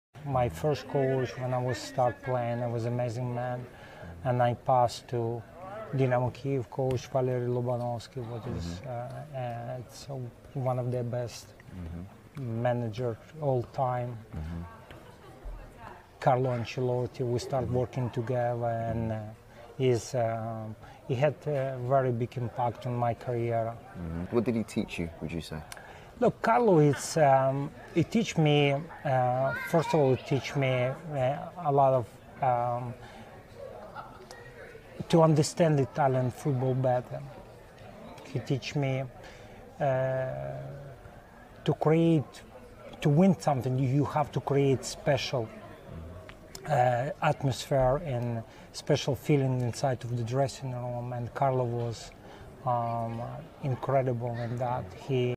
Andriy Shevchenko about Carlo Ancelotti's impact on his career
Andriy Shevchenko discusses the Best Players At AC Milan whilst playing under Carlo Ancelotti, could it be Kaka, Pirlo, Seerdorf, Inzaghi or Crespo? The 2004 Ukrainian Ballon D'or winner also spoke about his time at Chelsea under Jose Mourinho with great players such a Frank Lampard, John Terry, Didier Drogba Makelele. The failure to win the Champions League against Liverpool and coming close under Avram Grant. Andriy Shevchenko spoke to Joel Beya from FIVE and Cheeky Sport about his preference out of Kylian Mbappe and Erling Haaland. | Pochettino's Chances At Chelsea FC this season with new signings Mykhailo Mudryk, Nicolas Jackson, Christopher Nkunku, Enzo Ferndandes.

Game 4 Ukraine provides an opportunity for fans to put their rivalries aside and come together for one day with one.